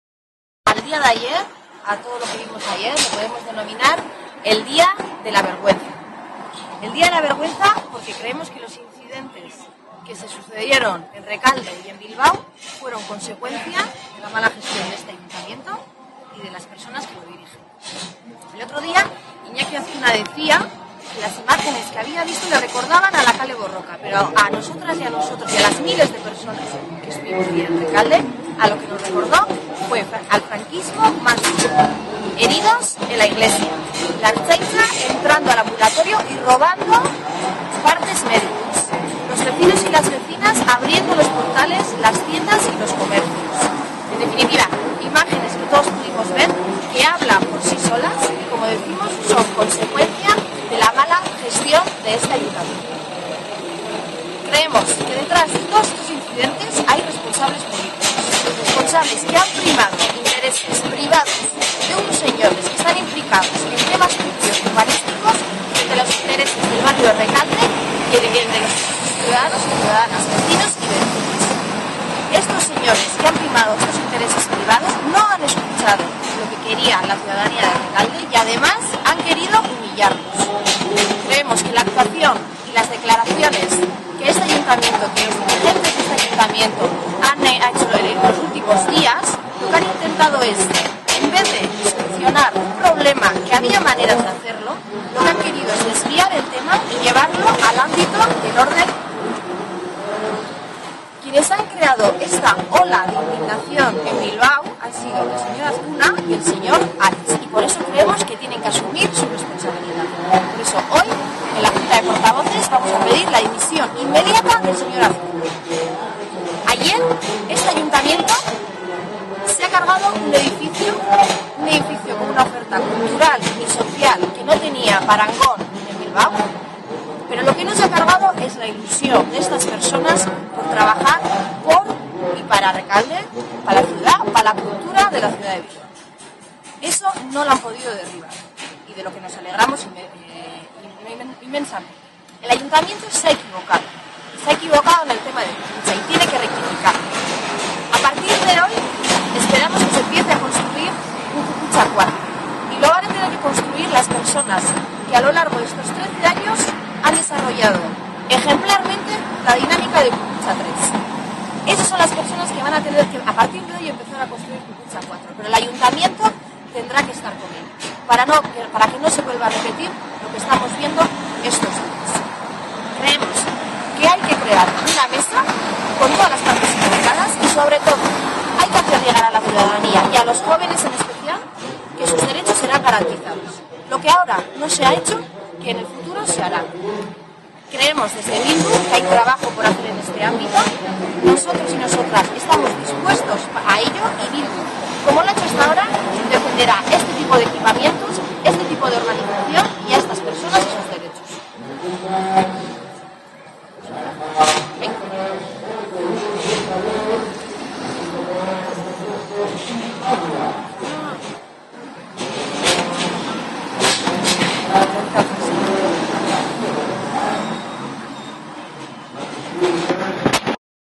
Bildu solicita la dimisión de Iñaki Azkuna por la gestión del conflicto de Kukutza
La concejala de Bildu Aitziber Ibaibarriaga solicita la dimisión del alcalde de Bilbao Iñaki Azkuna, por su nefasta gestión del conflicto de Kukutza